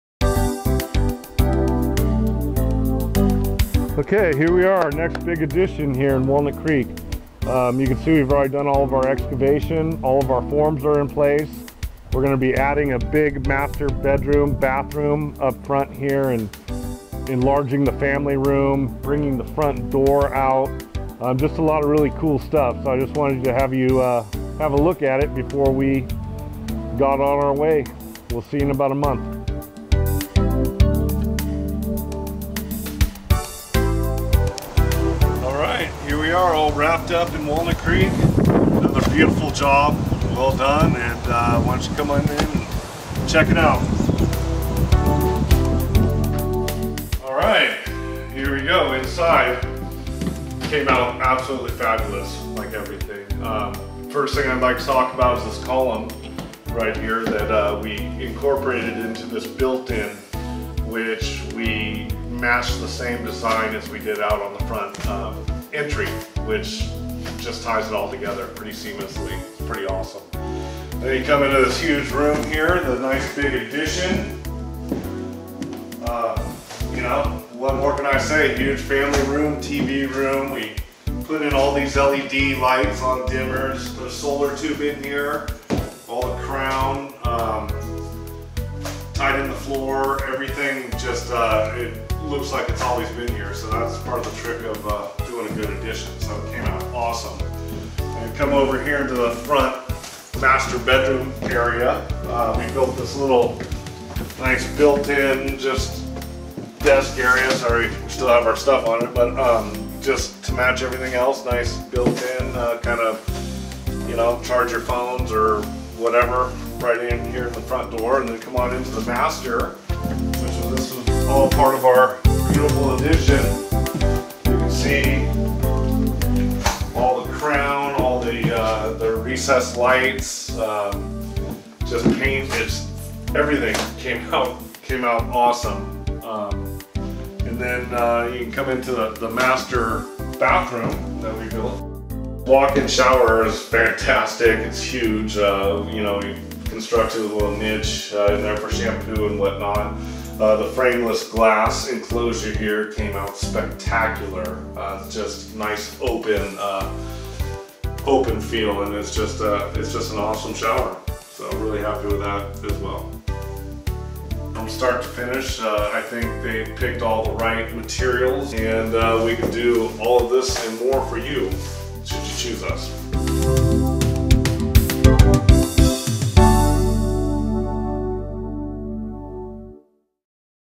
Decade Construction, another awesome addition
Robert Leitz of Decade Construction shows the BEFORE and AFTER of the fabulous additions his team made to a house in Walnut Creek, California.  They created a wonderful new master bedroom and bathroom, and they also expanded in improved a large family room.  As Robert says, the result as always with their work is "awesome."